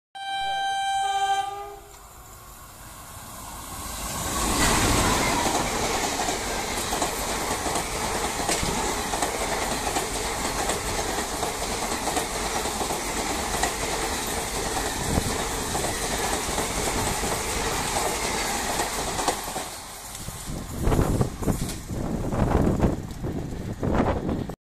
12333 Howrah Pryagraj Vibhuti SF Express 110 kmph Highly Speed Skip Kuchman Railway Station
12333 Howrah Prayahraj Vibhuti SF Express 110 kmph Highly Speed Skip Kuchman Railway Station